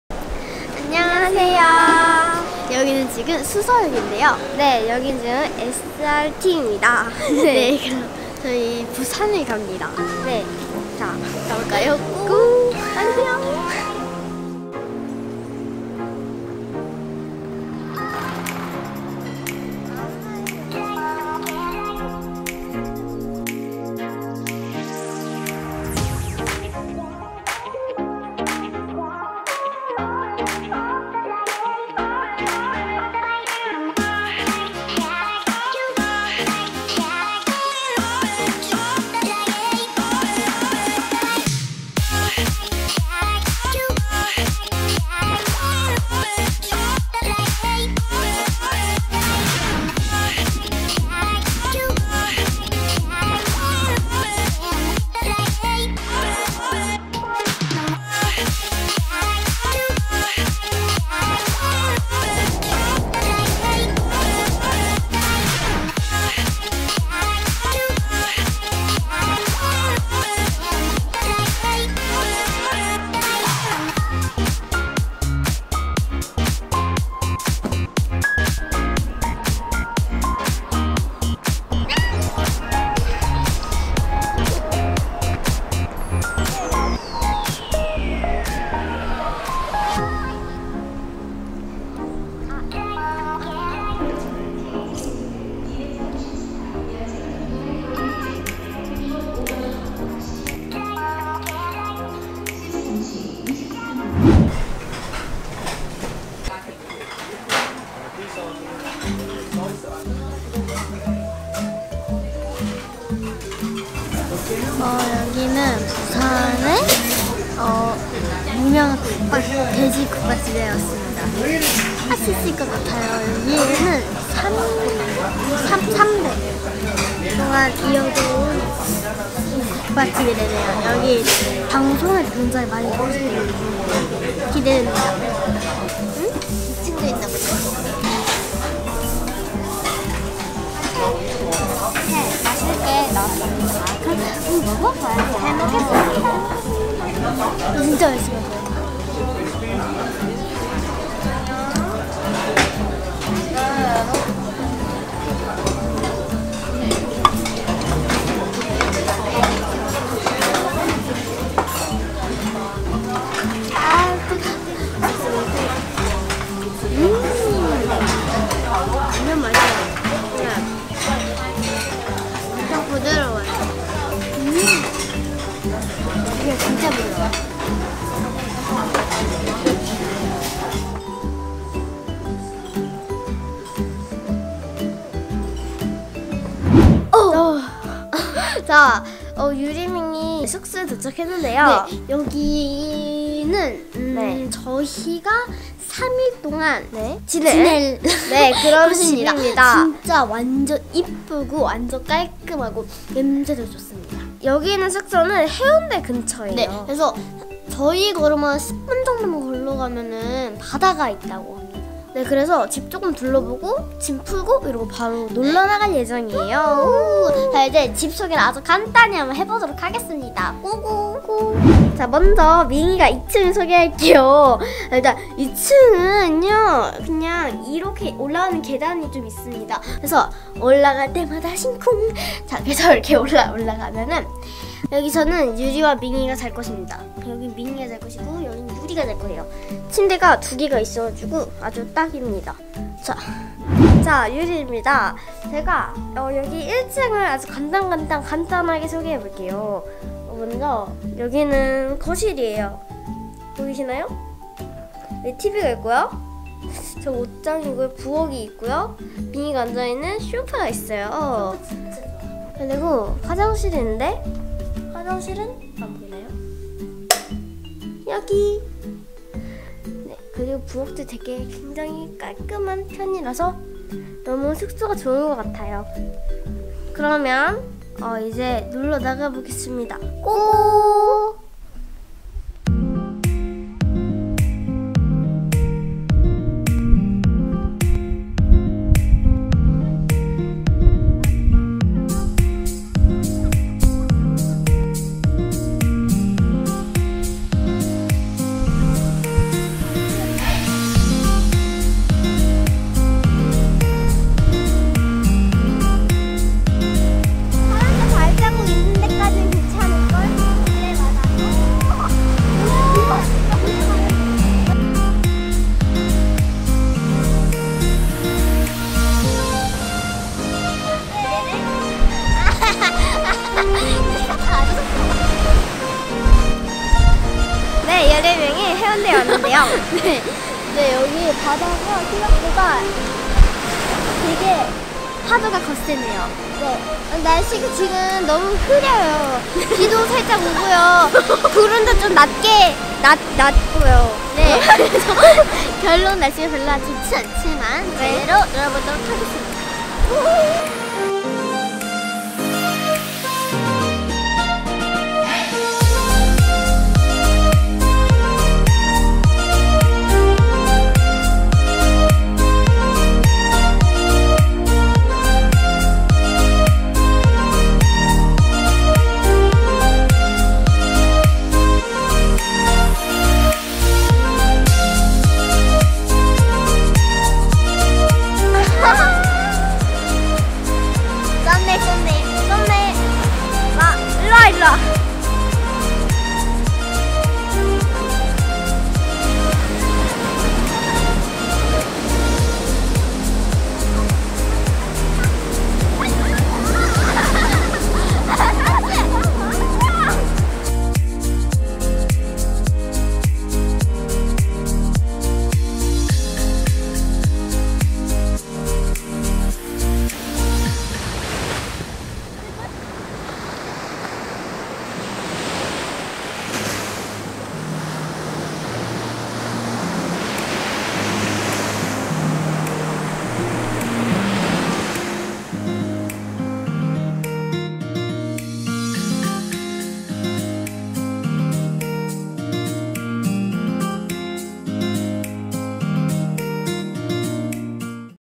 부산 왔어요!!🍓부산 Busan 여행 첫날~ 돼지국밥,숙소 소개,해운대 ♡ VLOG #018 ♡ [YuriMing 유리밍]
♡♡유리밍의 VLOG #018♡♡

안녕! ♡
유리밍 이예요~

기다리고 기다리던 단기 봄방학!!
짧은 단기 방학에 유리밍이 부산으로 여행을 떠납니다~

2박 3일 동안 여기저기 구경도 하고 맛있는 것도 먹고.
서울 수서역에서 SRT 고속 기차를 타고 갔어요.
기차여행이죠^^

부산역에 내려서 서면으로 이동!
'맛있는 녀석들'에서 나왔던 '송정3대국밥'집으로 출동!!
부슬부슬 비가 내렸지만 뭐 상관없음.^^
부산 대표 먹거리 돼지국밥으로 든든히 배를 채우고,
숙소로 갔습니다.♡

숙소는 해운대 근처!
바로 짐 풀고 해운대 해수욕장으로 갔어요♡♡
비오는 날씨에 바닷가에서 한참을 논 유리밍!!
어두워 질때 까지 신나게 놀고 숙소로 돌아와 쉬었지요^^

내일은 과연? 무슨일이???

DAY 1 in Busan!
A trip to Busan in May 2018.